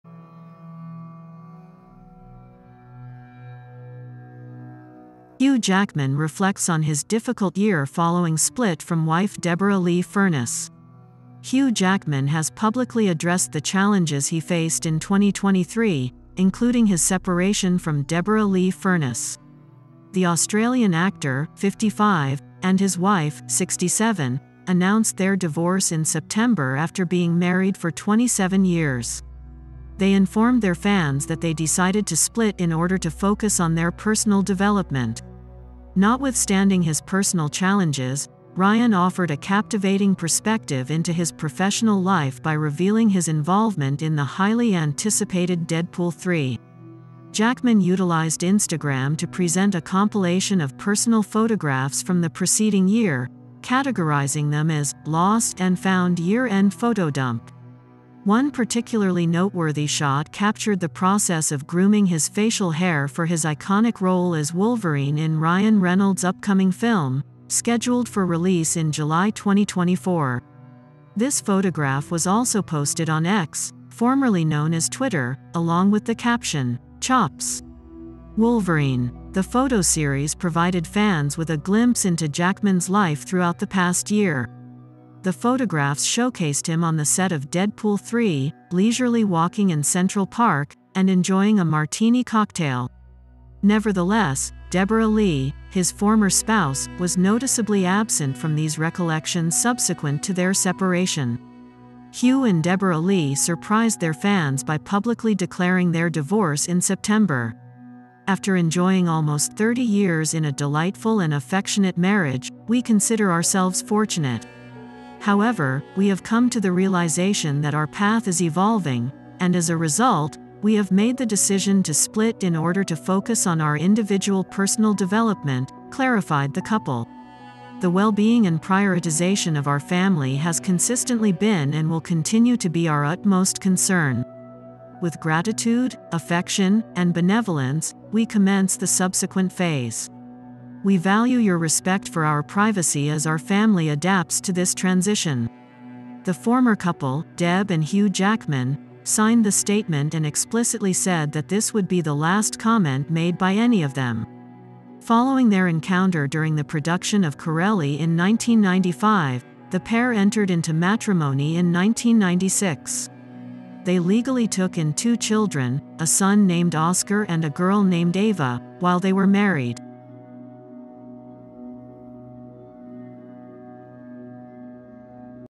Hugh Jackman reflects on his difficult year following split from wife Deborra-Lee Furness
The Australian actor, aged 55, and his wife, aged 67, announced their divorce in September after being married for 27 years. They informed their fans that they decided to split in order to focus on their personal development.